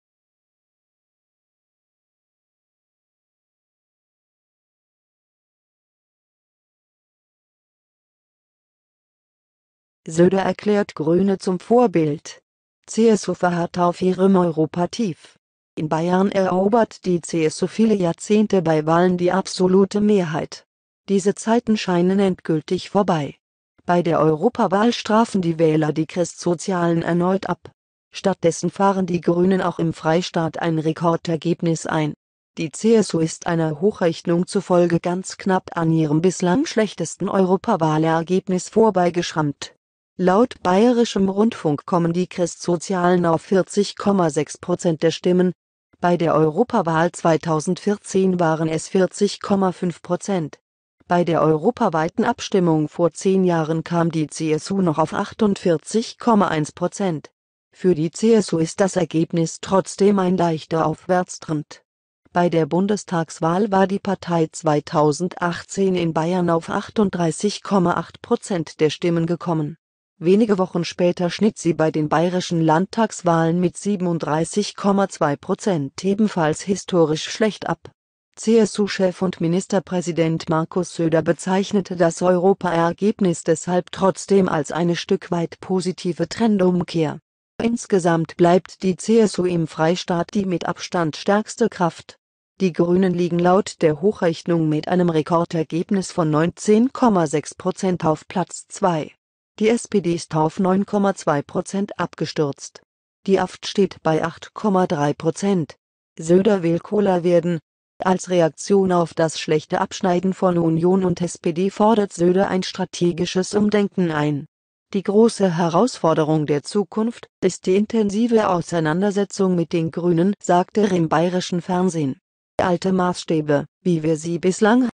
Söder erklärt Grüne zum Vorbild
CSU verharrt auf ihrem Europa-Tief.

In Bayern erobert die CSU viele Jahrzehnte bei Wahlen die absolute Mehrheit. Diese Zeiten scheinen endgültig vorbei: Bei der Europawahl strafen die Wähler die Christsozialen erneut ab. Stattdessen fahren die Grünen auch im Freistaat ein Rekordergebnis ein.

Die CSU ist einer Hochrechnung zufolge ganz knapp an ihrem bislang schlechtesten Europawahlergebnis vorbeigeschrammt. Laut Bayerischem Rundfunk kommen die Christsozialen auf 40,6 Prozent der Stimmen. Bei der Europawahl 2014 waren es 40,5 Prozent. Bei der europaweiten Abstimmung vor zehn Jahren kam die CSU noch auf 48,1 Prozent.

Für die CSU ist das Ergebnis trotzdem ein leichter Aufwärtstrend: Bei der Bundestagswahl war die Partei 2018 in Bayern auf 38,8 Prozent der Stimmen gekommen. Wenige Wochen später schnitt sie bei den bayerischen Landtagswahlen mit 37,2 Prozent ebenfalls historisch schlecht ab. CSU-Chef und Ministerpräsident Markus Söder bezeichnete das Europa-Ergebnis deshalb trotzdem als eine "Stück weit positive Trendumkehr".

Insgesamt bleibt die CSU im Freistaat die mit Abstand stärkste Kraft. Die Grünen liegen laut der Hochrechnung mit einem Rekordergebnis von 19,6 Prozent auf Platz zwei. Die SPD ist auf 9,2 Prozent  abgestürzt. Die AfD steht bei 8,3 Prozent.

Söder will "cooler" werden.

Als Reaktion auf das schlechte Abschneiden von Union und SPD fordert Söder ein strategisches Umdenken ein. "Die große Herausforderung der Zukunft ist die intensive Auseinandersetzung mit den Grünen", sagte er im Bayerischen Fernsehen. "Alte Maßstäbe, wie wir sie bislang hatten, gelten nicht mehr." Die SPD erwähnte der CSU-Chef in seiner Stellungnahme nicht. Söder ermahnte die CDU außerdem, "wieder jünger, cooler, offener zu werden. Wir müssen mit den Themen und der Kommunikation so agieren, dass wir nicht von gestern wirken."

Ähnlich äußerte sich auch der CSU-Landesgruppenchef im Bundestag, Alexander Dorbrindt. "Man sieht sehr deutlich jetzt auch bei den Umfragen zur Wahl, dass das Thema Klimaschutz eine ganz, ganz große Rolle gespielt hat", sagte er bei n-tv. "Da müssen wir auch stärker werden, diese Themen besser kommunizieren, stärker auch besetzen, wenn man da mehr Zustimmung haben will."

Bei der Europawahl waren 10,2 Millionen Menschen in Bayern wahlberechtigt. Damit weist der Freistaat nach Nordrhein-Westfalen die zweimeisten Wahlberechtigten in Deutschland auf.